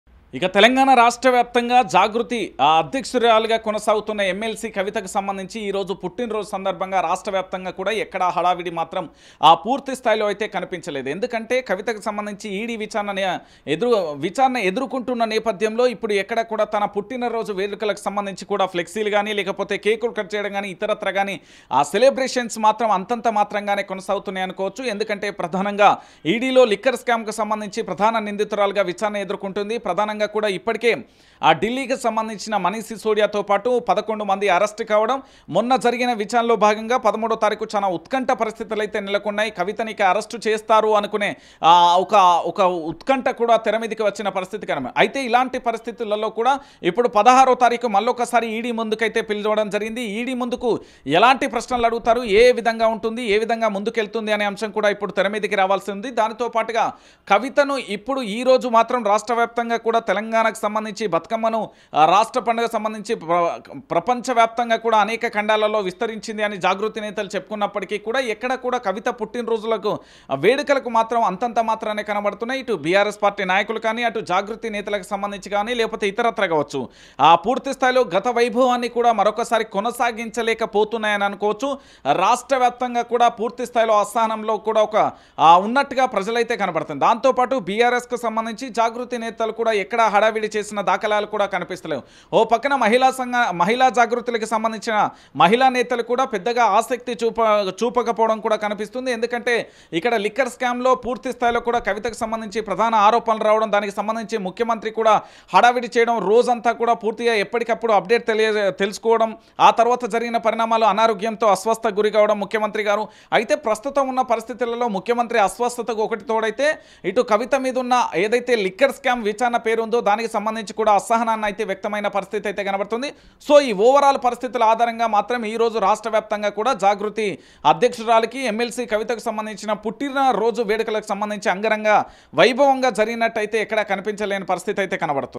తెలంగాణలో కవితను నమ్మే పరిస్థితి లేదు || జాగృతికి దూరం కానున్న మహిళలు | YR TV Telugu
తెలంగాణలో కవితను నమ్మే పరిస్థితి లేదు జాగృతికి దూరం కానున్న మహిళలు